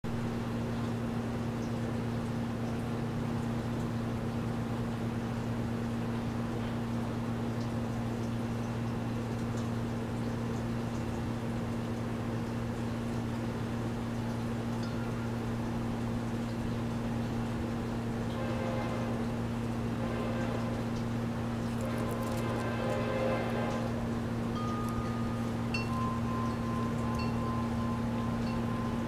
'Lizzy G' - Training for the Circus!.MTS
Lizzy is a blue-bellied lizard and lives in my yard. She's eager to learn new tricks which earns her tasty mealworms;-)